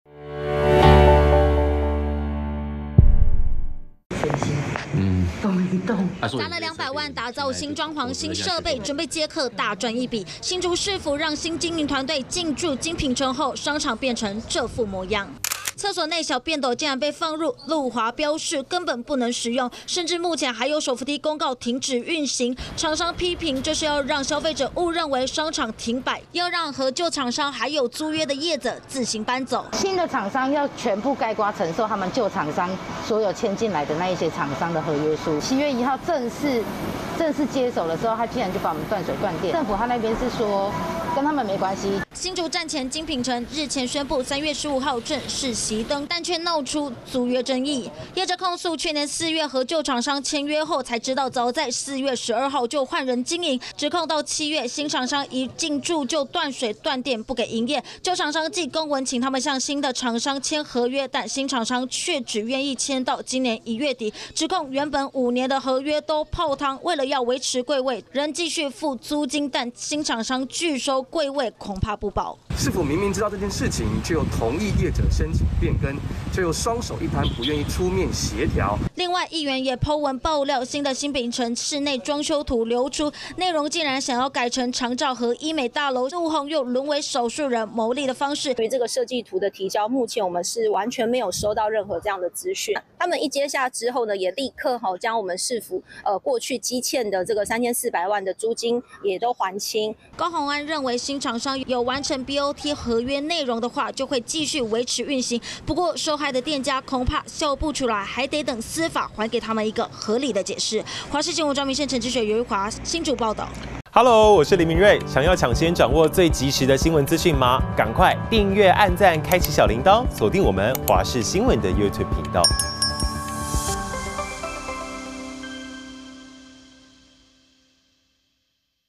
控斷水電.廁所堆雜物 晶品城業者批廠商不照合約｜華視新聞 20240312
新竹站前晶品城將在15日正式熄燈，傳出去年7月就有新廠商進駐，但仍然有部分櫃位和舊廠商仍有租約，這些業者指控事後大樓有些出口不能進出，廁所被堆放雜物，甚至被斷水斷電，批評是想讓消費者誤認商場停擺，要讓剩餘業者自行搬走，而議員也指出新曝光的室內裝修圖，竟然要改成醫美跟長照大樓，質疑難道是高虹安復甦舊城商圈的解方嗎？對此，新竹市長高虹安則回應市府沒收到裝修圖。